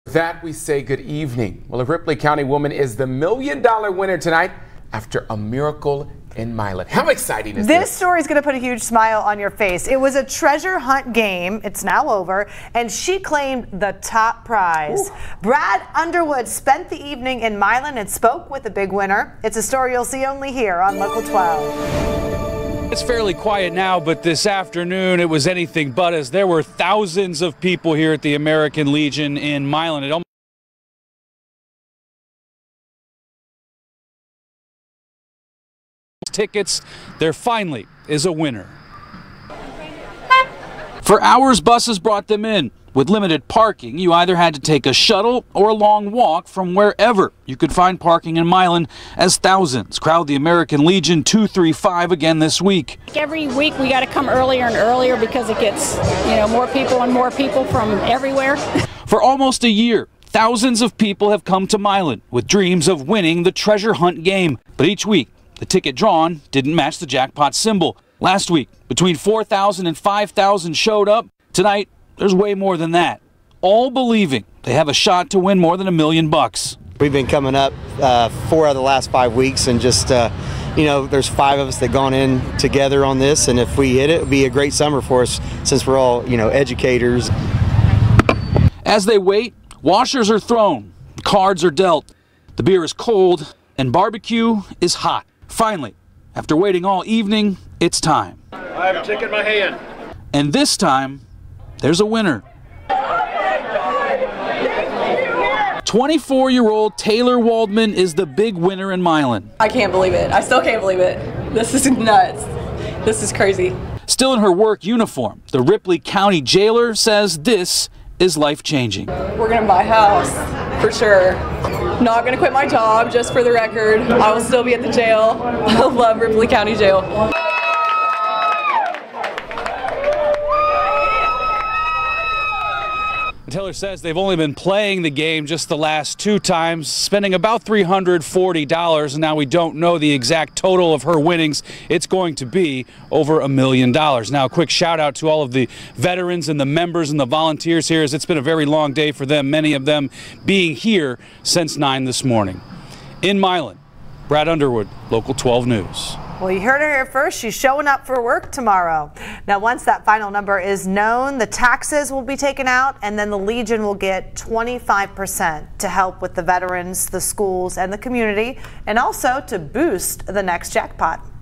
Indiana woman wins over $1 million in Milan treasure hunt game
The Tri-State area has a new millionaire as of Monday -- 24-year-old Taylor Waldman